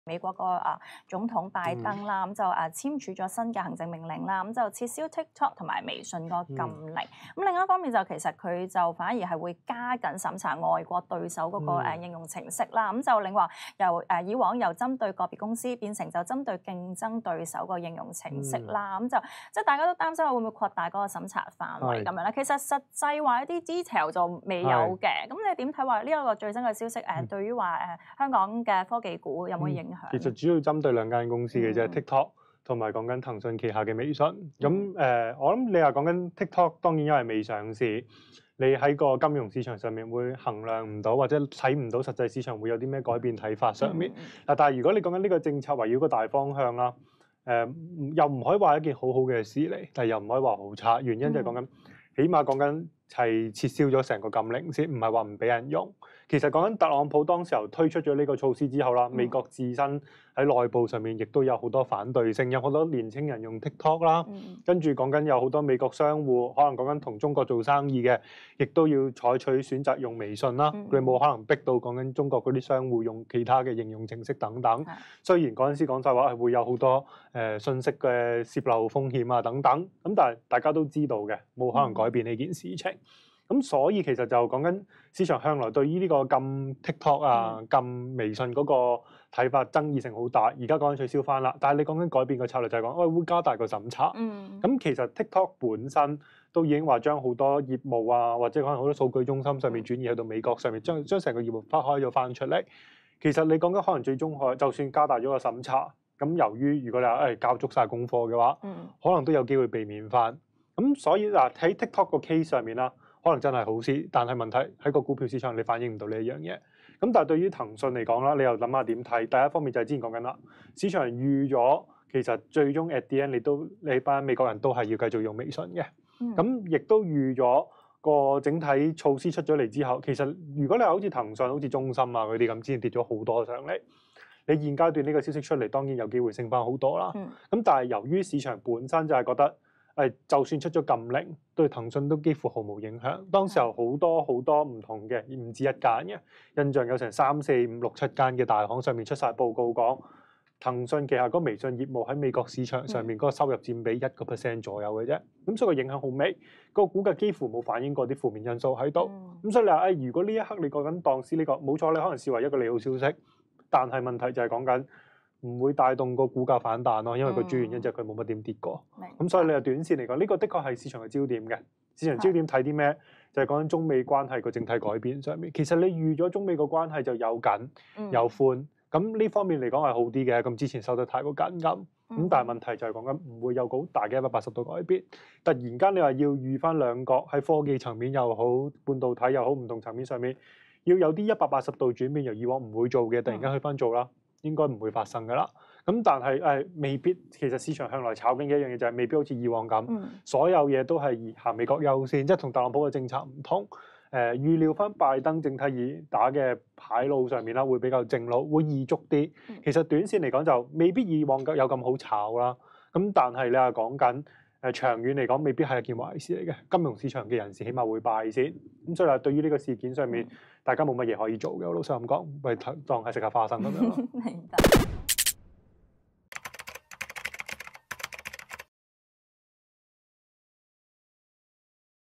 【ET開市直擊】（精點）美國撤銷Tik Tok微信禁令 對科技股有何影響
美國總統拜登簽署新行政命令，撤銷前任美國總統特朗普對字節跳動旗下TikTok 和微信下達的禁令，如何解讀是項政策對金融市場影響。